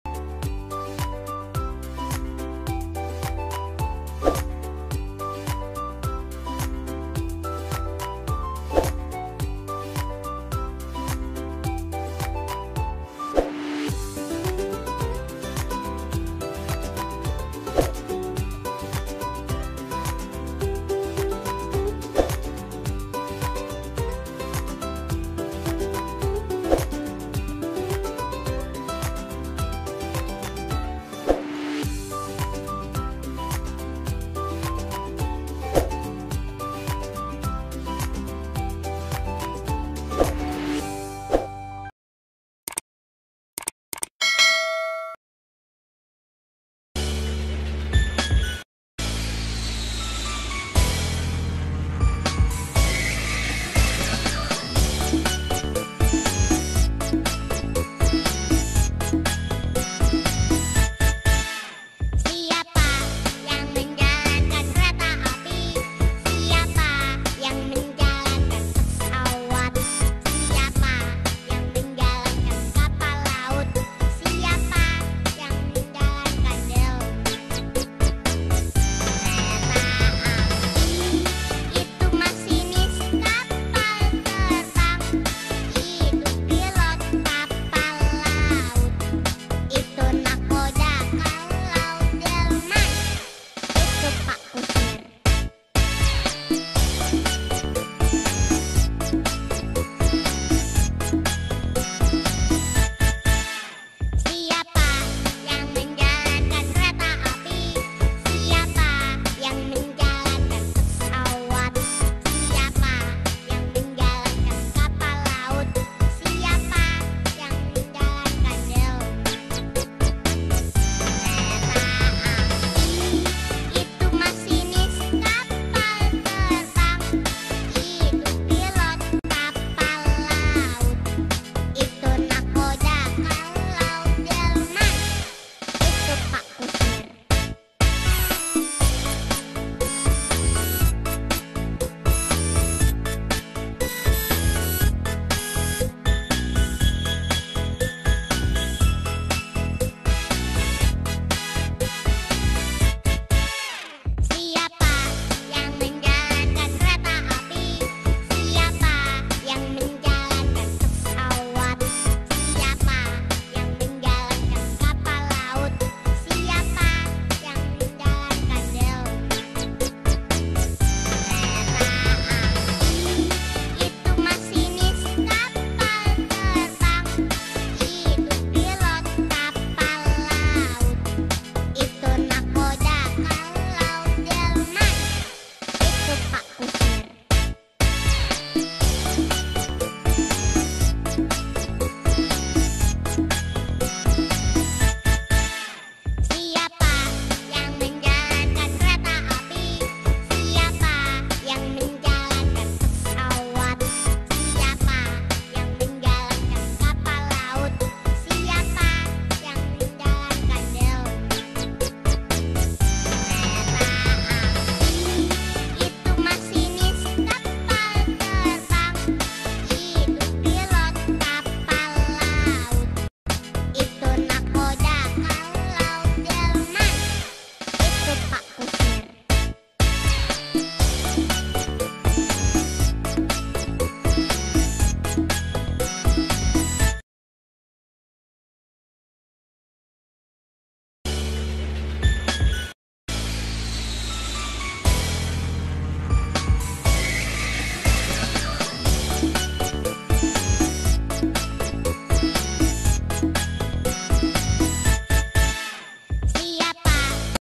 SENAM PENGEMUDI KENDARAAN ( Senam Anak PAUD / TK Tema Kendaraan / Profesi )
Assalamu'alaikum semuanya 💕
Kali ini aku buat video SENAM PENGEMUDI KENDARAAN yang sering dipakai di PAUD / TK , senam ini bisa juga dipakai saat sedang pembelajaran tema kendaraan ataupun profesi. Yuk senam bareng aku supaya sehat 😊

Terimakasih banyak support dari kalian semua buat aku semangat untuk terus buat video 😊